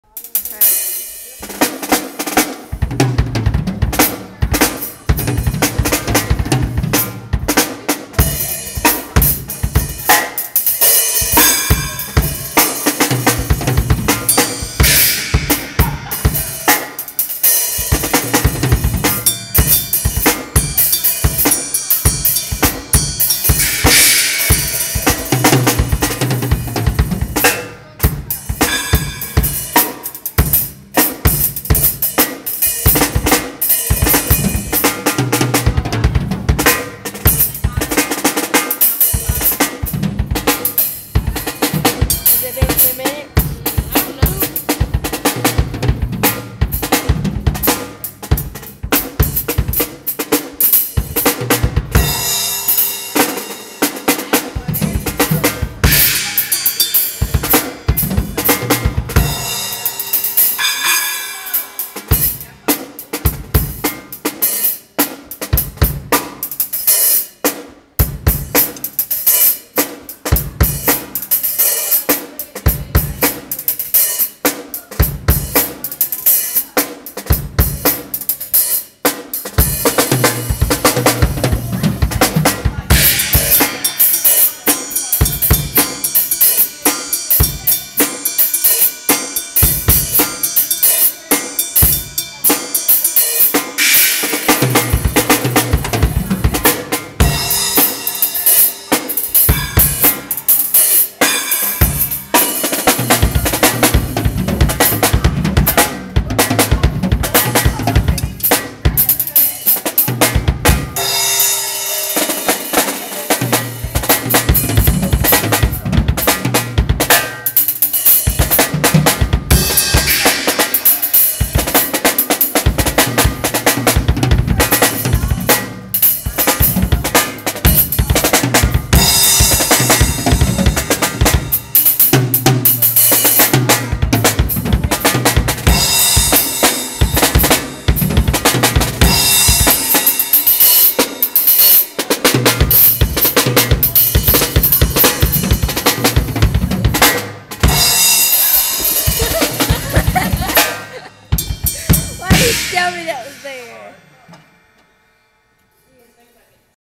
Fresh P on the Drums
Marcus Working on his druming skills!